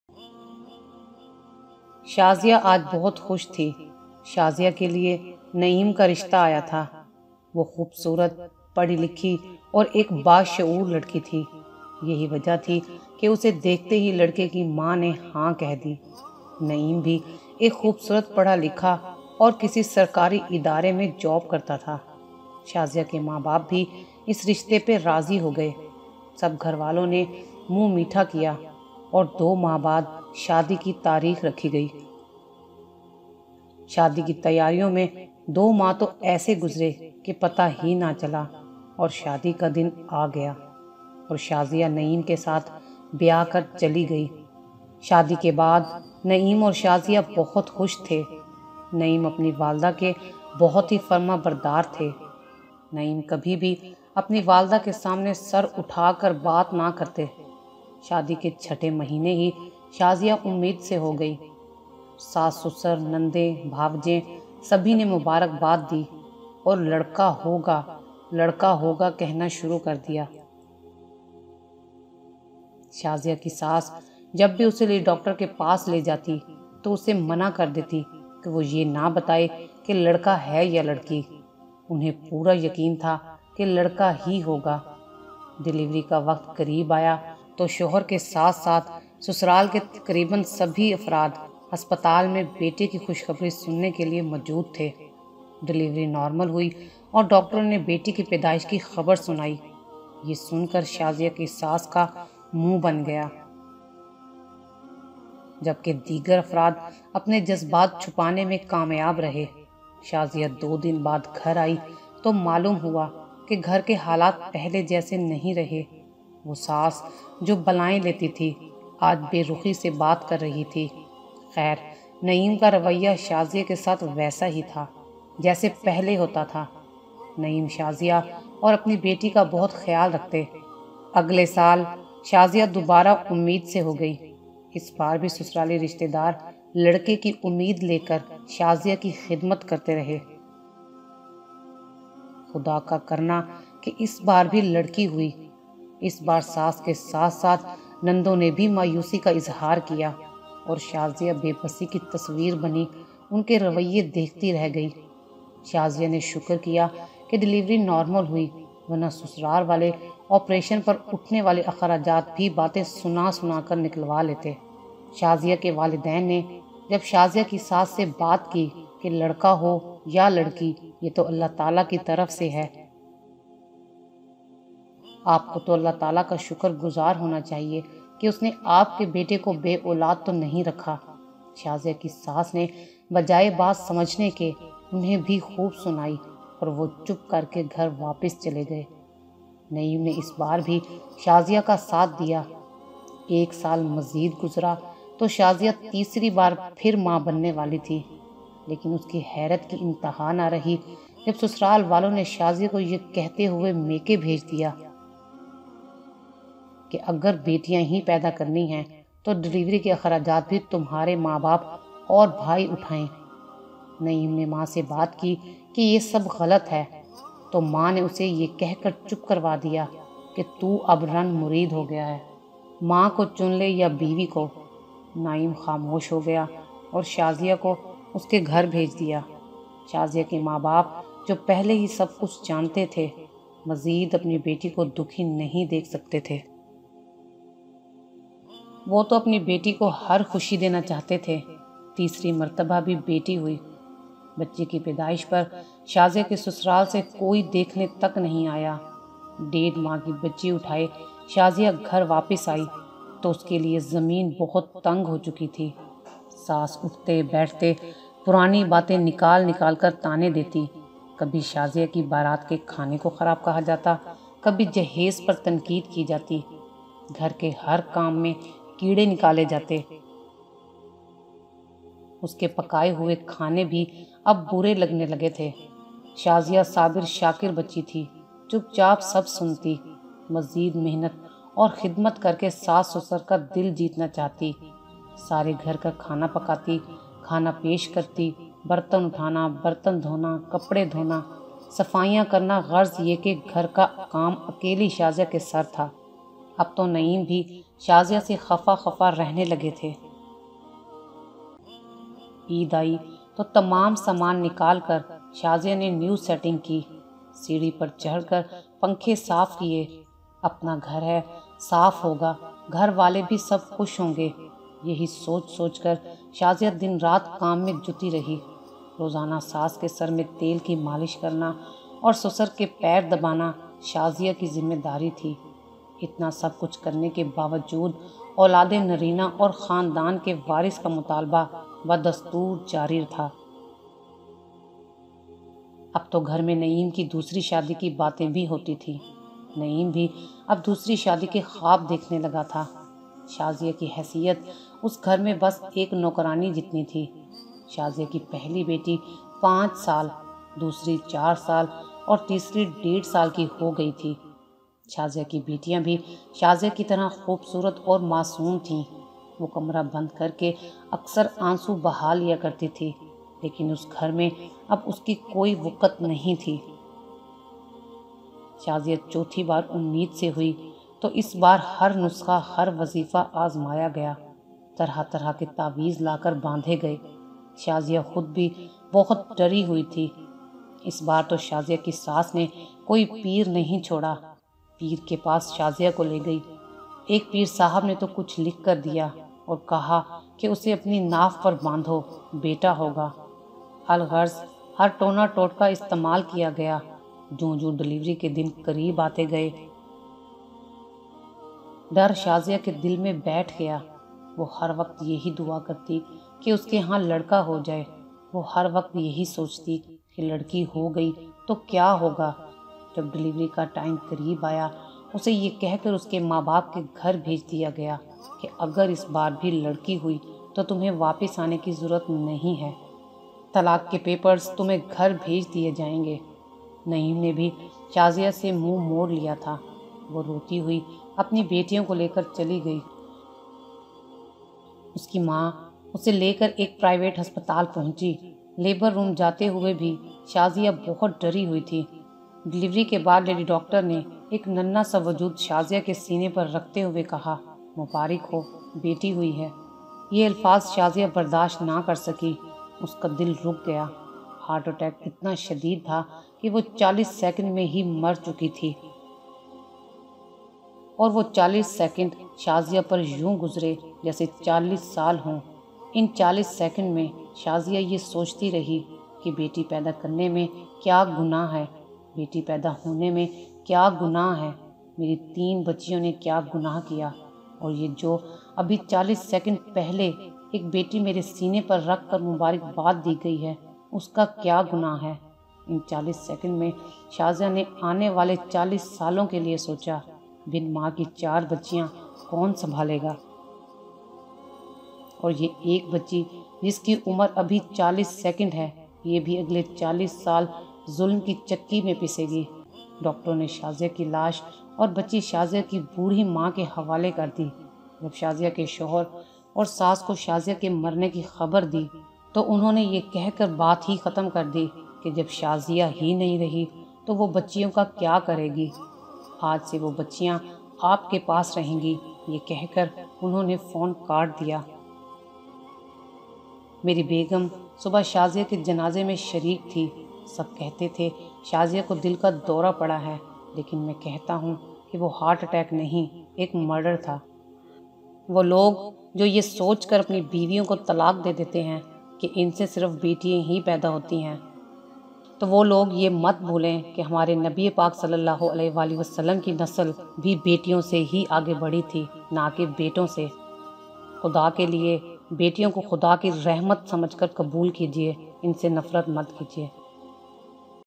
Shazia ki Saas aur aik Peer Ki rula deny wali Kahani || New Sachi Kahani # 31
Shazia ki Saas aur ek Peer Ki rula deny wali Kahani || New Sachi Kahani # 31